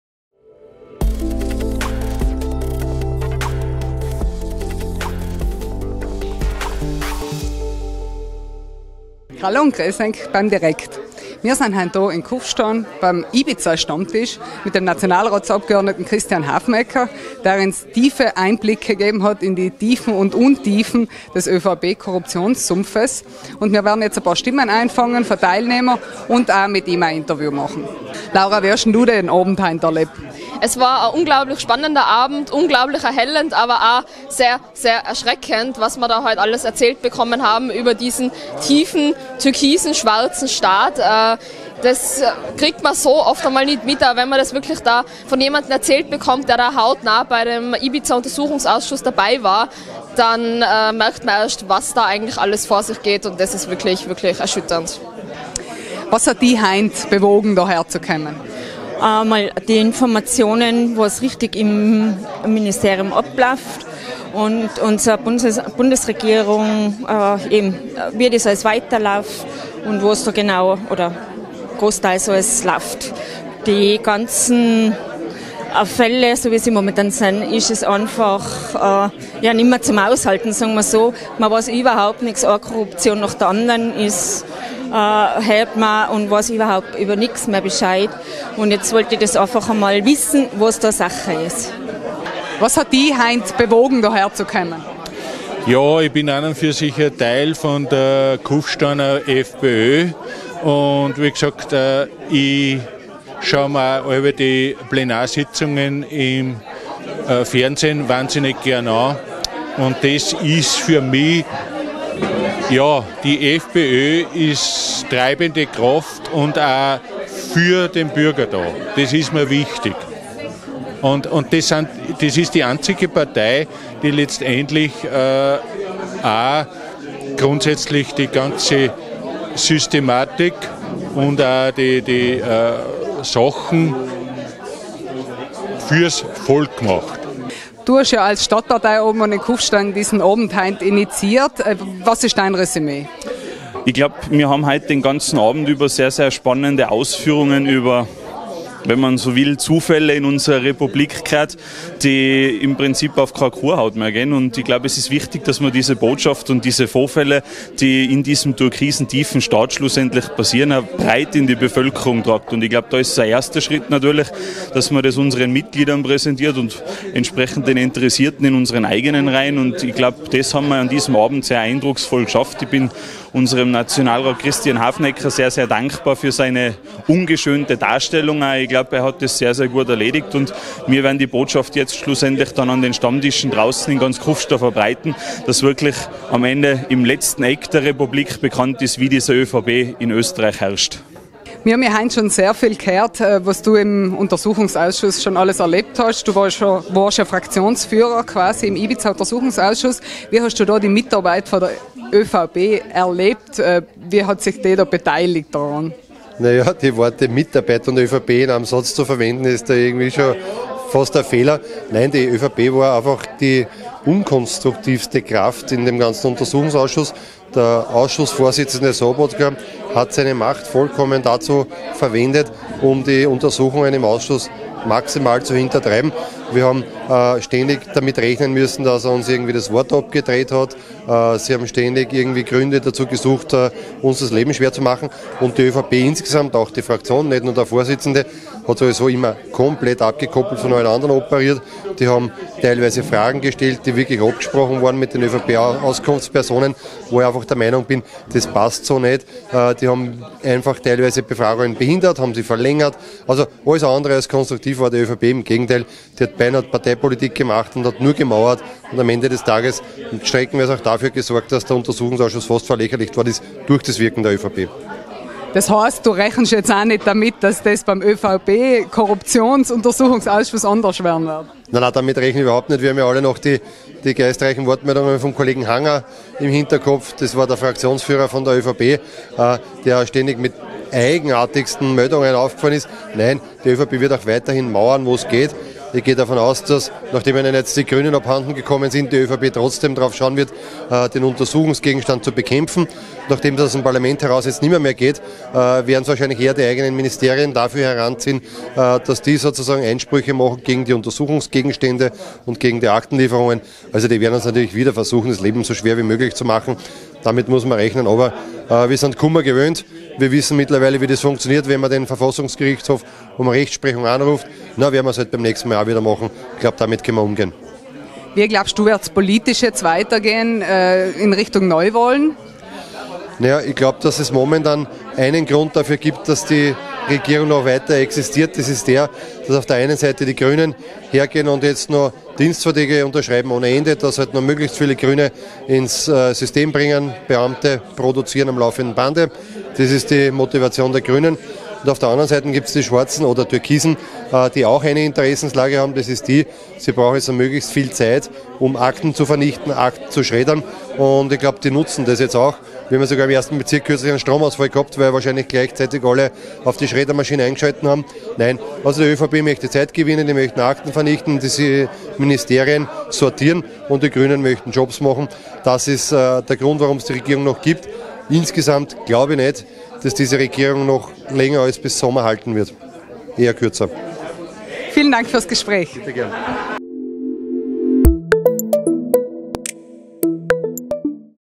Direkt | 24.10.2021 | Stammtisch der FPÖ Kufstein zum Ibiza-Untersuchungsausschuss
Am vergangenen Dienstag war der freiheitliche Fraktionsführer im Ibiza-Untersuchungsausschuss, Christian Hafenecker, zu Gast in Kufstein und informierte einige Interessierte über die Vorgänge in der Republik rund um die Ibiza-Affäre und die daraus resultierenden Ermittlungen gegen einige führende ÖVP-Funktionäre.